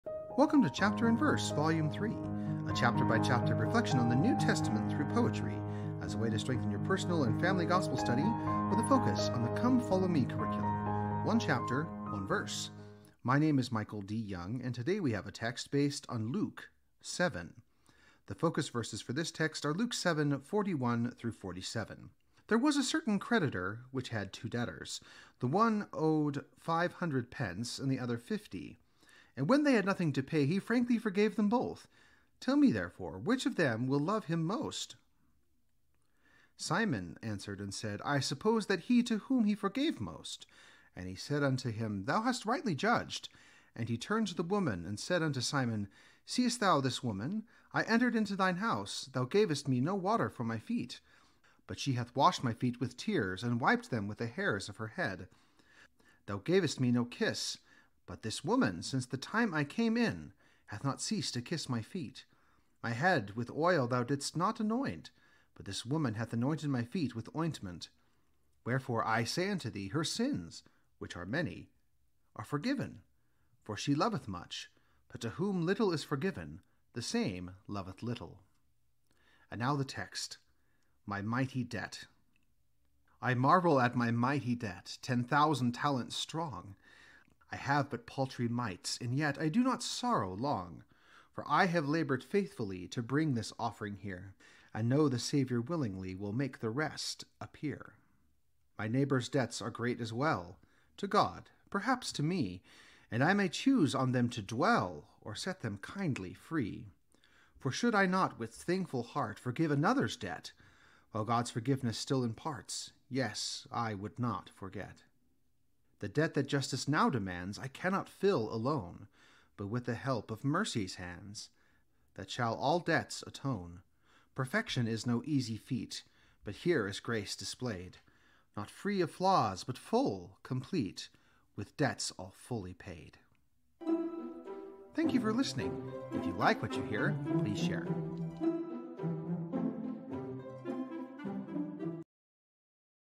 Luke 7
Welcome to Chapter and Verse, Volume III, a chapter by chapter reflection on the New Testament through poetry, as a way to strengthen your personal and family Gospel study with a focus on the Come, Follow Me curriculum.

One chapter, one verse.

My name is Michael D. Young and today we have a text based on Luke 7.